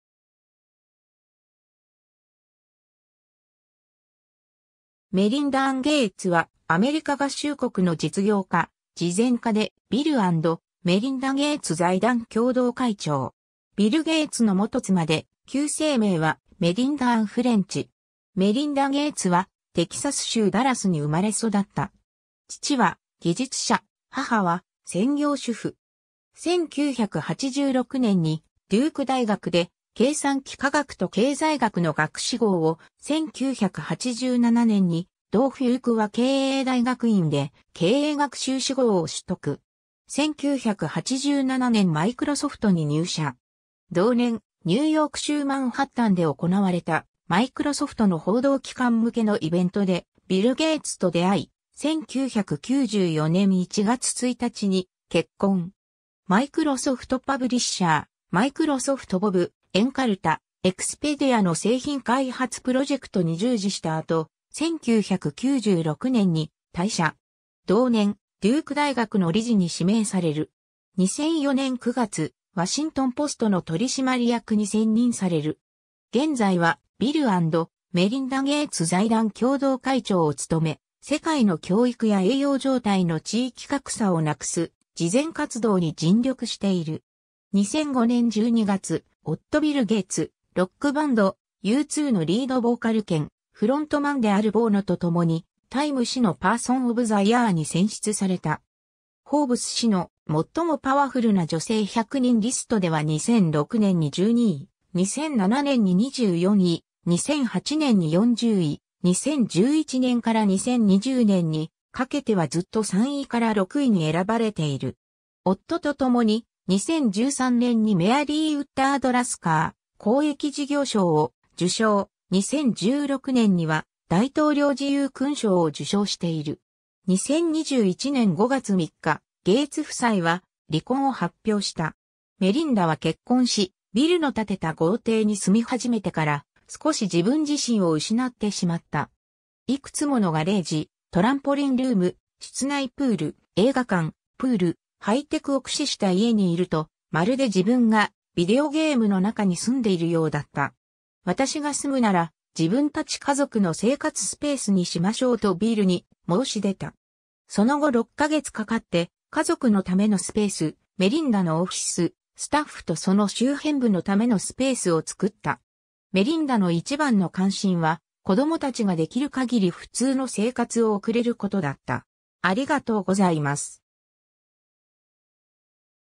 メリンダ・ゲイツ
タイム誌が選ぶパーソン・オブ・ザ・イヤー
ビル・ゲイツ
ワシントン・ポストの人物
メリンダ・アン・ゲイツ（Melinda Ann Gates, 1964年8月15日 - ）は、アメリカ合衆国の実業家、慈善家で、ビル&メリンダゲイツ財団共同会長。
ビル・ゲイツの元妻で、旧姓名はメリンダ・アン・フレンチ（Melinda Ann French）。
 メリンダ・ゲイツはテキサス州ダラスに生まれ育った。
父は技術者、母は専業主婦。
1986年にデューク大学で計算機科学と経済学の学士号を、1987年に同フュークワ経営大学院で経営学修士号を取得。
 1987年（23歳）マイクロソフトに入社。
同年、ニューヨーク州マンハッタンで行われたマイクロソフトの報道機関向けのイベントでビル・ゲイツと出会い、1994年1月1日に結婚。
Microsoft Publisher、Microsoft Bob、エンカルタ、Expedia の製品開発プロジェクトに従事した後、1996年に退社。
同年、デューク大学の理事に指名される（2003年まで）。
2004年9月、ワシントン・ポストの取締役に選任される。
 現在はビル&メリンダゲイツ財団共同会長を務め、世界の教育や栄養状態の地域格差をなくす慈善活動に尽力している。
2005年12月、夫ビル・ゲイツ、ロックバンドU2のリードボーカル兼フロントマンであるボノと共に『タイム』誌のパーソン・オブ・ザ・イヤーに選出された。
『フォーブス』誌の「最もパワフルな女性100人」リストでは2006年に12位、2007年に24位、2008年に40位、2011年～2020年にかけてはずっと3位～6位に選ばれている。
 夫と共に、2013年にメアリー・ウッダード・ラスカー公益事業賞を受賞、2016年には大統領自由勲章を受章している。
 2021年5月3日、ゲイツ夫妻は離婚を発表した。
 メリンダは結婚し、ビルの建てた豪邸に住み始めてから、少し自分自身を失ってしまった。
いくつものガレージ、トランポリンルーム、室内プール、映画館、プール、ハイテクを駆使した家にいると、まるで自分がビデオゲームの中に住んでいるようだった。
「私が住むなら、自分達家族の生活スペースにしましょう」とビルに申し出た。
その後6ヶ月かかって、家族のためのスペース、メリンダのオフィス、スタッフとその周辺部のためのスペースを作った。
メリンダの一番の関心は、子供達が出来る限り普通の生活を送れることだった。